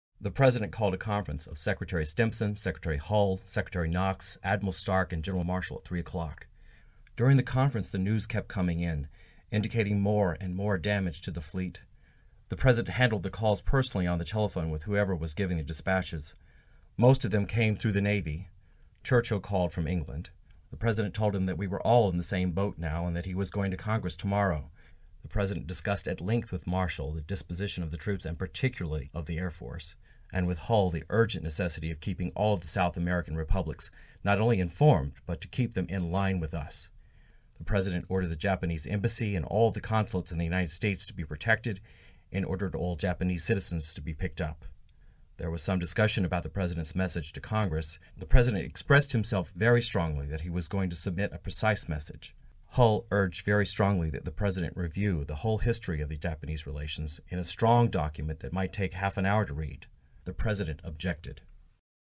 EYEWITNESS TO HISTORY - December 7, 1941 War Conference - Harry Hopkins
EYEWITNESS TO HISTORY - December 7, 1941
Harry Hopkins, Adviser and friend
Listen to excerpts (read by actor)
Hopkins was with FDR for most of December 7. That night he described Roosevelt’s war conference in a long personal memo.
Citation: Memorandum, December 7, 1941 in Georgetown University Library, Special Collections Research Center